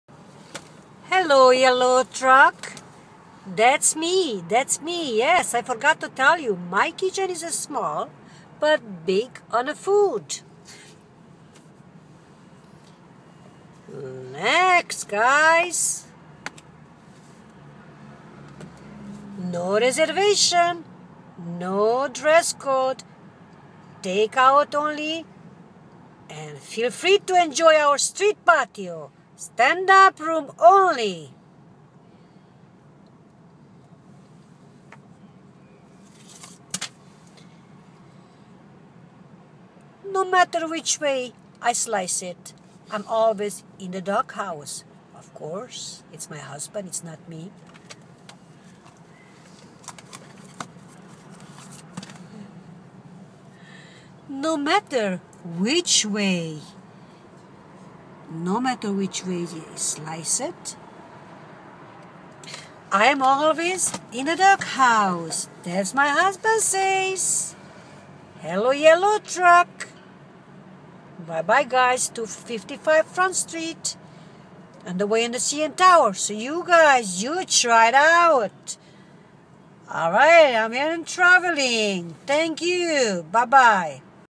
Hello-yellow truck good food
Canadian flag on hockey stick
For 27 years same location simcoe/front street (255 front street in front of intercontinental hotel)
Cooking with canola oil everyday fresh.
Very good food and good price
Try it and you will become a lover of Canadian food
Small kitchen we love all we are the first original vender
-3